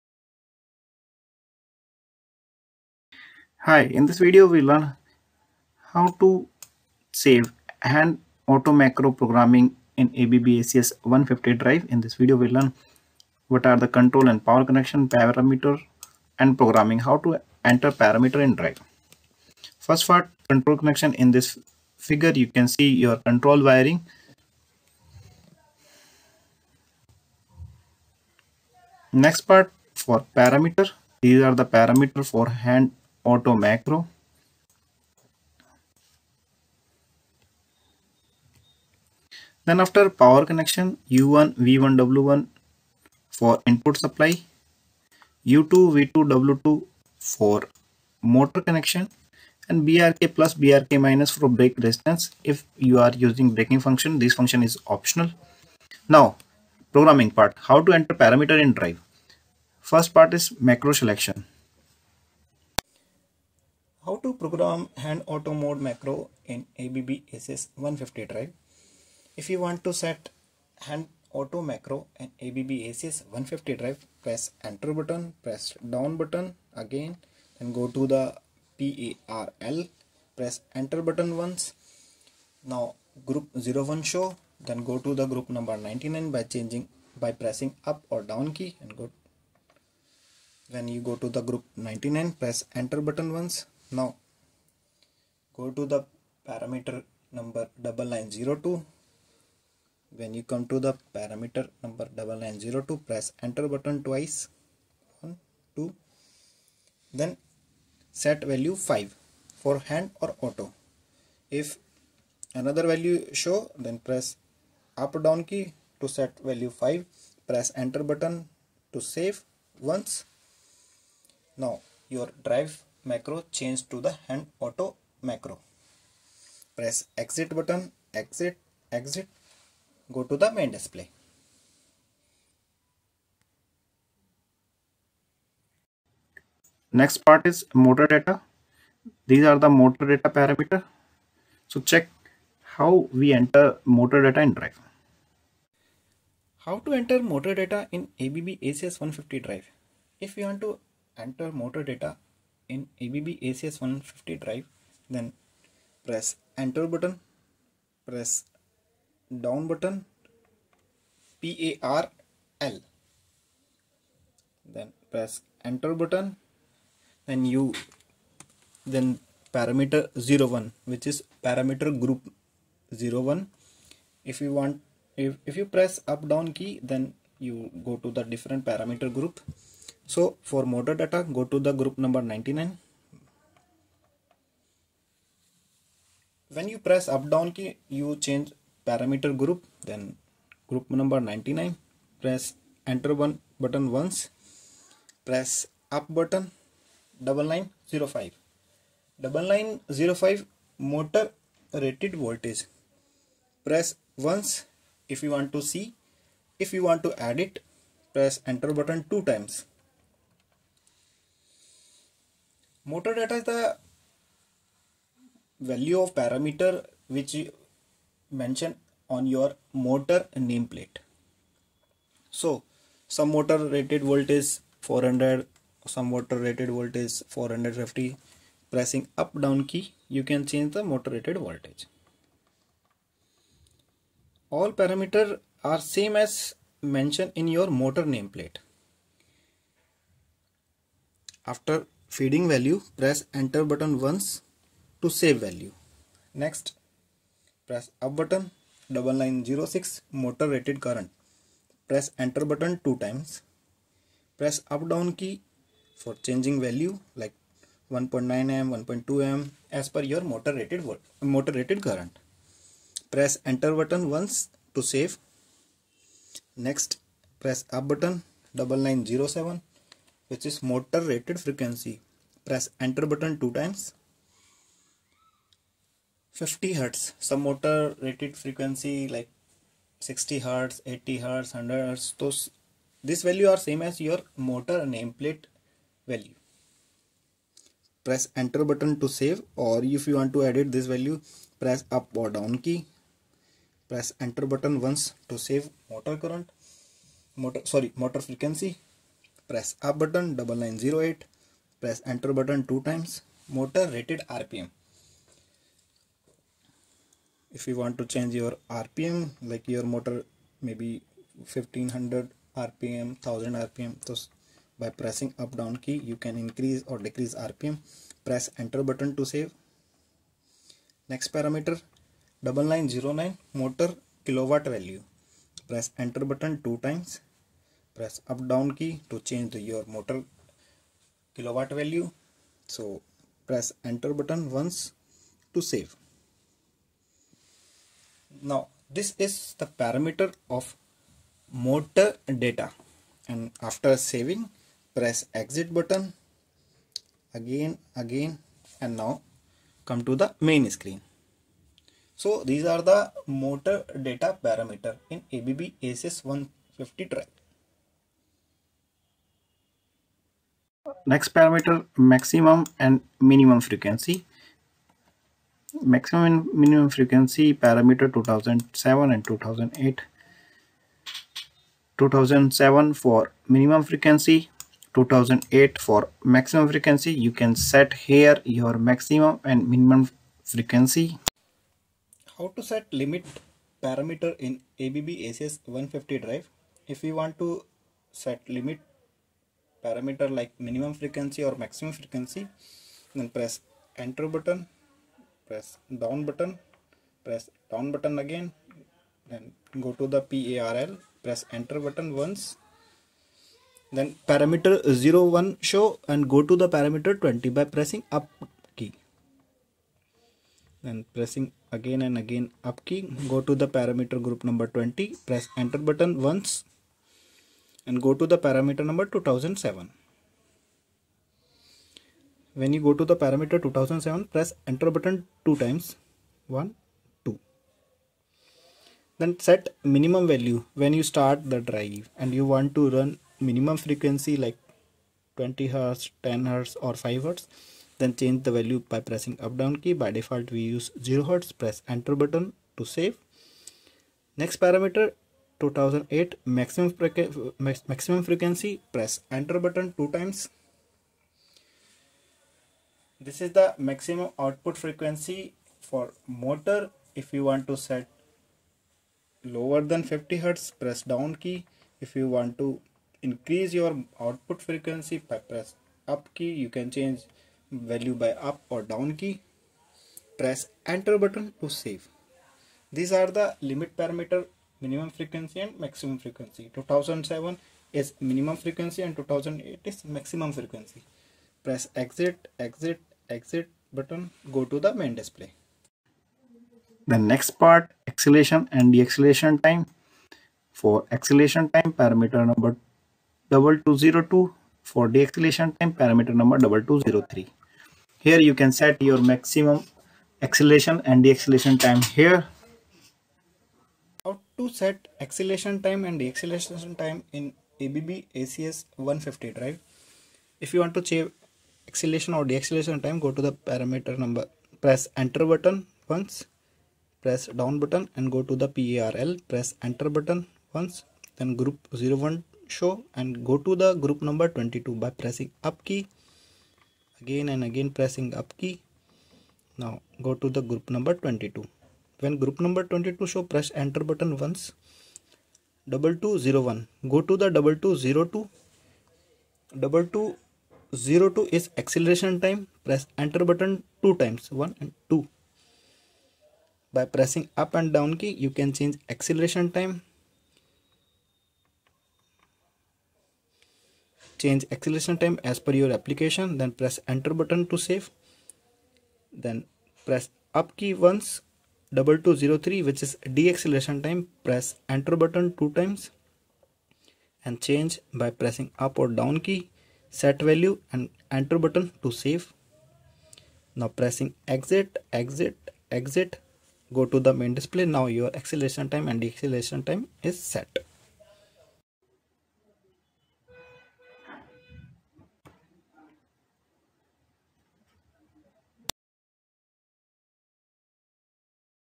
ABB ACS150 Drive Hand/Auto Macro Programming in English
In this video we show hand auto macro programming with complete parameter and wiring and also how to enter parameter in drive with complete detail.